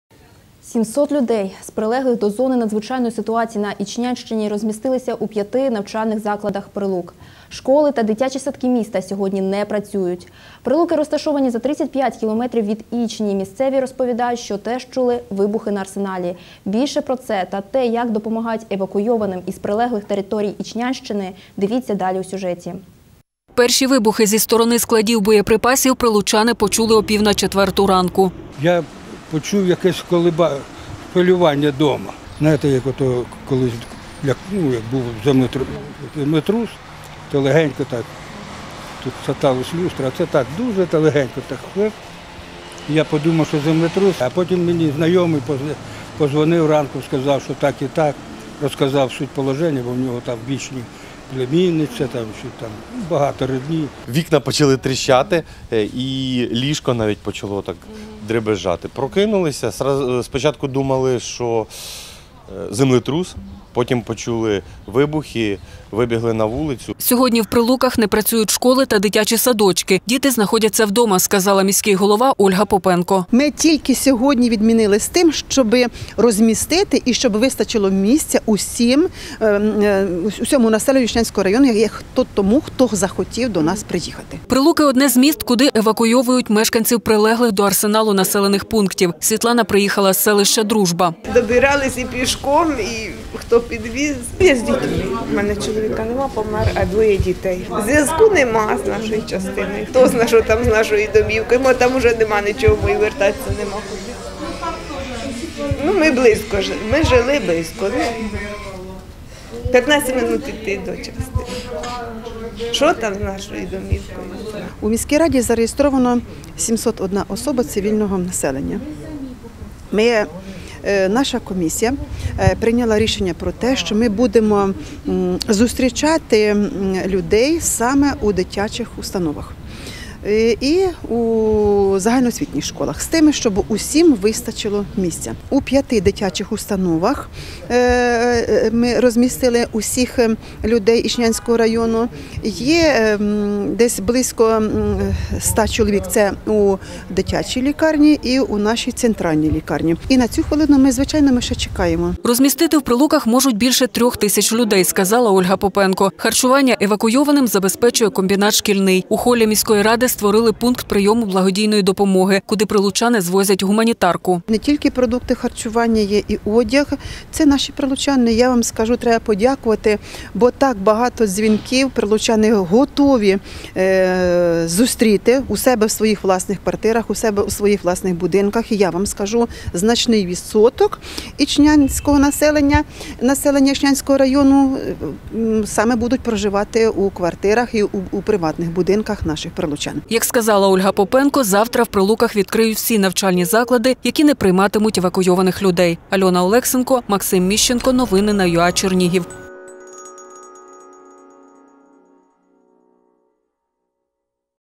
У Прилуках у 5 навчальних закладах розмістили понад 700 евакуйованих людей
Школи і дитячі садки сьогодні не працюють у Прилуках. Там, у 5 навчальних закладах, розмістили понад 700 людей, евакуйованих з сіл поблизу ічнянських складів боєприпасів. Прилуки розташовані за 35 кілометрів від місця вибуху. Сьогодні вночі містяни чули вибухи на арсеналі. Деталі – у сюжеті наших прилуцьких колег.

Перші вибухи зі сторони складів боєприпасів прилучани почули опівначетверту ранку.

Григорій Петрович, прилучанин: «Я почув таке коливання у квартирі, так наче трохи як землетрус, я і подумав, що землетрус, а тоді знайомий позвонив, шо мов, так і так, він там в ічні живе.
Дмитро Пахомов, прилучанин. Вікна почали тріщати і ліжко навіть почало дребежжать. Прокинулися сначала думали шо землетрус. Потім почули вибухи вибігли на вулицю.»

Сьогодні у Прилуках не працюють школи та дитячі садочки, діти знаходяться вдома, сказала міський голова Ольга Попенко.

Ольга Попенко, Прилуцький міський голова: «Ми відмінили тільки через те, щоб було місце розмістити населення Ічнянського району, хто до нас приїхав.»

Прилуки – одне з міст, куди евакуйовують мешканців прилеглих до арсеналу населених пунктів. Світлана приїхала з селища Дружба.

Світлана: «Ми пішком ішли і хто підвіз. Звязку немає з нашою частиною. Хтозна шо там. Може уже і немає нашої домівки, бо ми близько живем, 15 минут до частини.»

Ольга Попенко: «На зараз зареєстровано 701 людина. Ми їх розмістили у 5 установах. Дитячих садках та дитячій лікарні – у кого діти. І дорослій лікарні. Ми всім забезпечені. На зараз ще чекаємо людей.»

Розмістити у Прилуках зможуть більше 3 тисяч людей, сказала Ольга Попенко. Харчування евакуйованим забезпечує комбінат «Шкільний».
У холлі міської ради створили пункт прийому благодійної допомоги, куди прилучани звозять гуманітарку.

Ольга Попенко: «Тут і продукти і одяг, і прилучани, треба їм подякувати, готові приймати людей у себе в квартирах. І значний відсоток ічнянців буде жити у домівках прилучан.»

Як сказала Ольга Попенко, завтра у Прилуках відкриють всі навчальні заклади, які не прийматимуть евакуйованих людей.